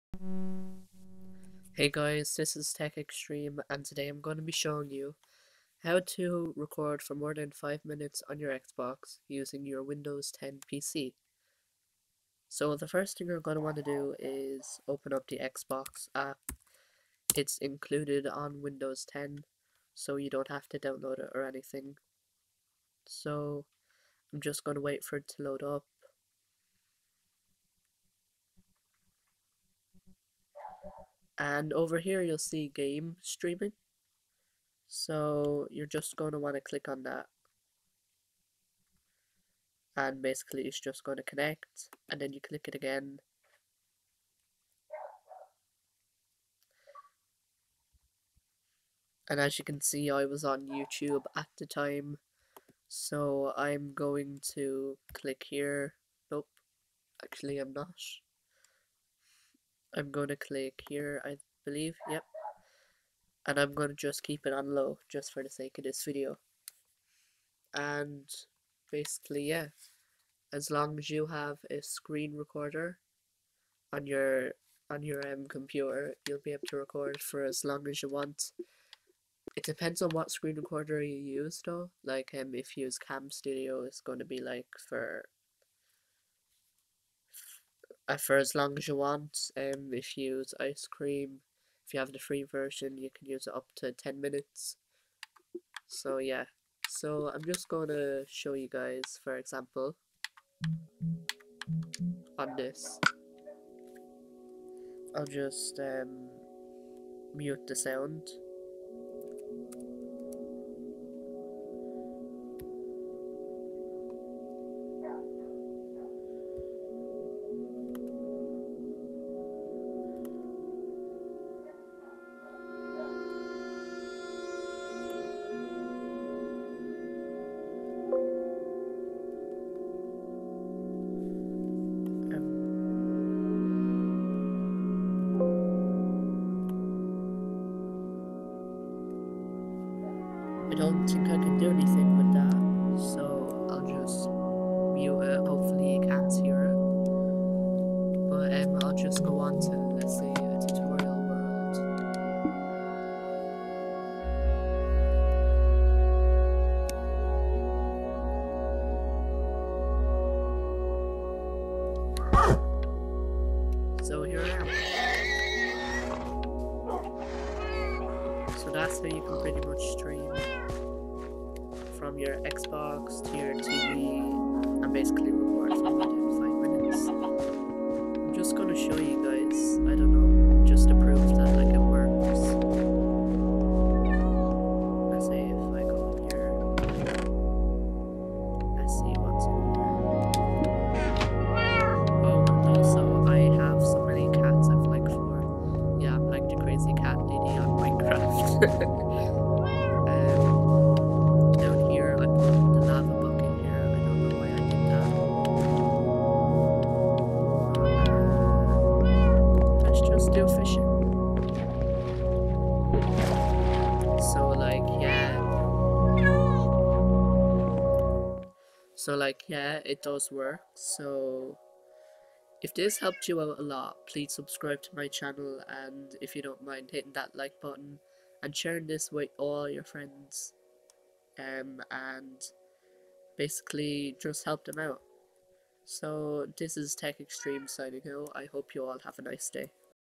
How to record for more than five minutes on your Xbox!
Hi Guys! I hope this helped all of you out as nobody has actually shown a proper way on how to do this and now, there is a way to do it without a glitch, trick or costing you hundreds for a capture card!